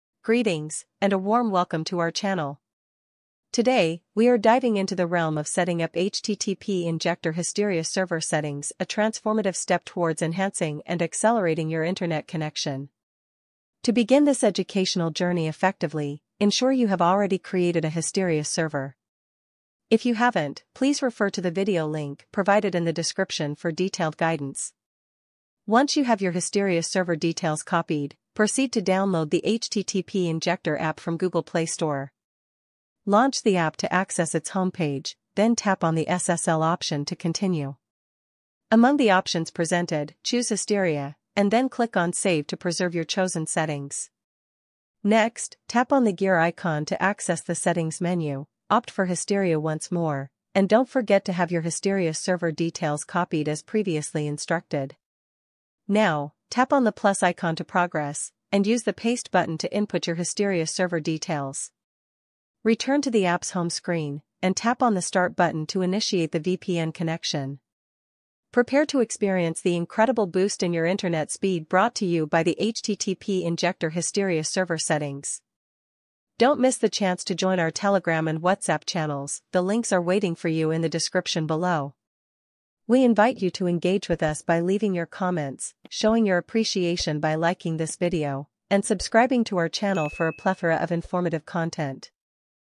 Boost Your Browsing Speed: Step-by-Step HTTP Injector Hysteria Server Setup
Enhanced Online Experience with HTTP Injector Hysteria Server Settings

In this enhanced tutorial, we guide you through setting up HTTP Injector with Hysteria Server settings to significantly improve your internet connection. This optimized setup allows you to boost your browsing speed and enhance overall internet performance.

To get started, you’ll first need to create a Hysteria server and obtain the necessary server details. Once you’ve done that, download the HTTP Injector app from the Google Play Store. After entering your Hysteria server information, activate these settings within the app for an enhanced internet experience, ensuring smoother and faster browsing with minimal disruptions.